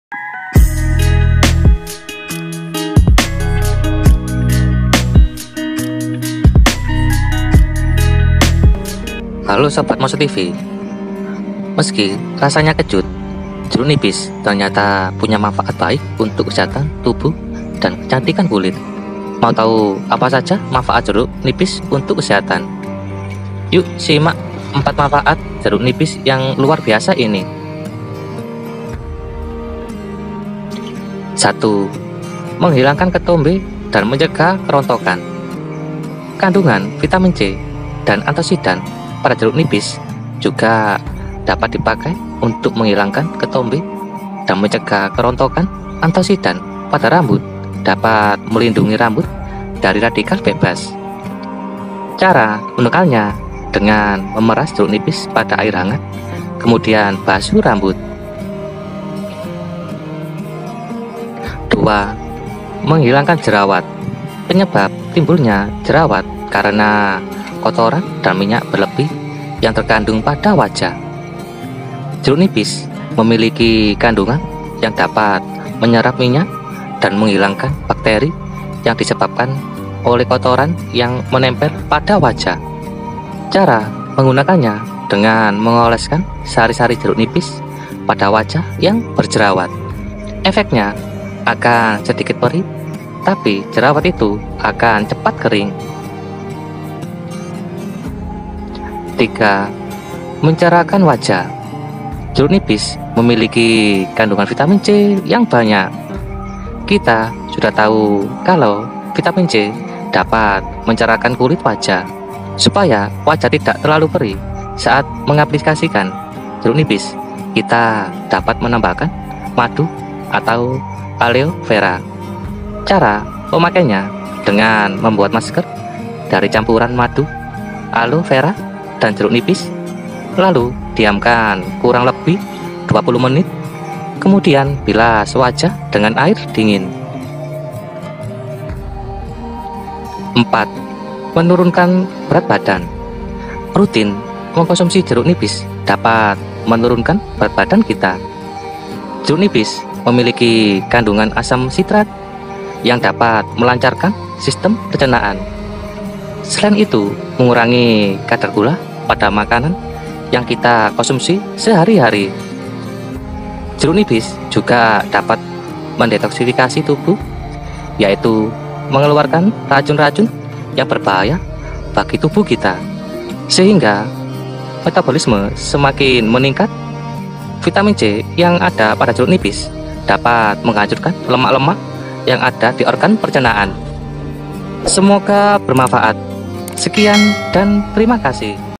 Ternyata Ini Fakta Sebenarnya Manfaat Jeruk Nipis untuk Kecantikan dan Kesehatan
Kali ini saya akan membahas fakta sebenarnya tentang beberapa manfaat dan khasiat jeruk nipis untuk kesehatan dan kecantikan yang harus anda tahu untuk belajar menjaga kesehatan jeruk nipis ini sangat penting sobat moxer tv.